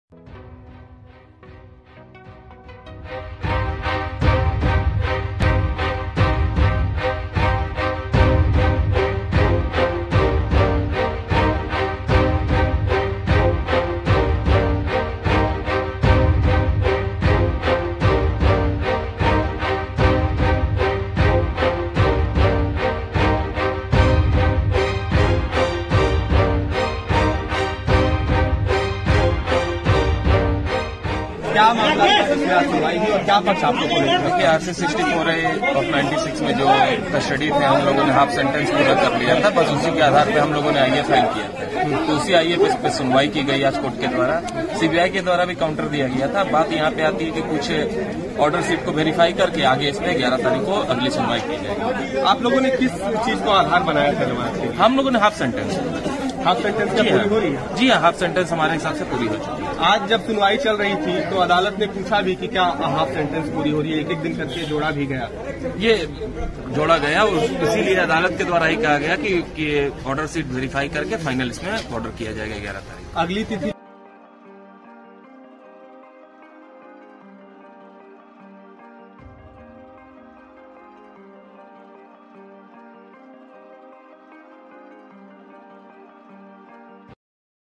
Lalu Prasad Yadav की जमानत याचिका पर सुनवाई टली,11 दिसंबर को होगी अगली सुनवाई
लालू प्रसाद यादव की जमानत याचिका पर रांची हाईकोर्ट में सुनवाई टली... आरजेडी अध्यक्ष की जमानत याचिका पर 11 दिसंबर को होगी सुनवाई.